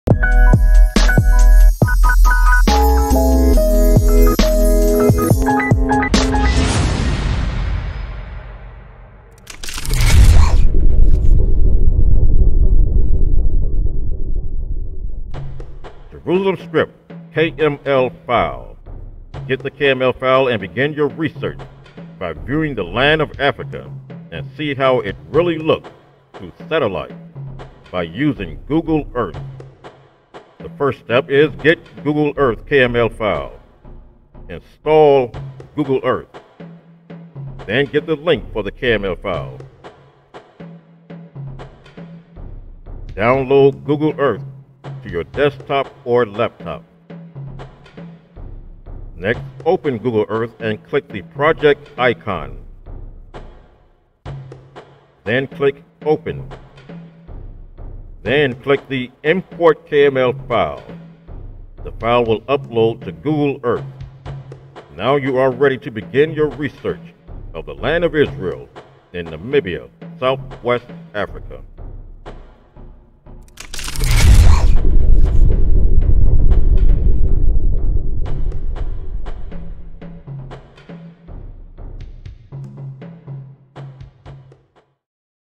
Jerusalem Script KML File
Jerusalem Script KML File, get the KML File and begin your research by viewing the land of Africa and see how it really look through satellite, by using Google Earth.

1.Get Google Earth
2. Install Google Earth
3. Download Google Earth to your desktop or laptop
4. Get the link for the KML File and Download Link
5. Open Google Earth and click the Project Icon
6. Click OPEN
7. Click Import KML File from Computer
8. The File will upload to Google Earth
9. Begin your research of the Land of Israel in Namibia Southwest Africa.

The KML File is a file that is editable and viewers can edit, add, delete, and update the file with information from their own research of the land of Israel in Namibia. The KML File will be updated periodically as new research data will be found and placed in the File. Get the KML File and begin your research of the land of Israel in Namibia Southwest Africa in the Karas Region.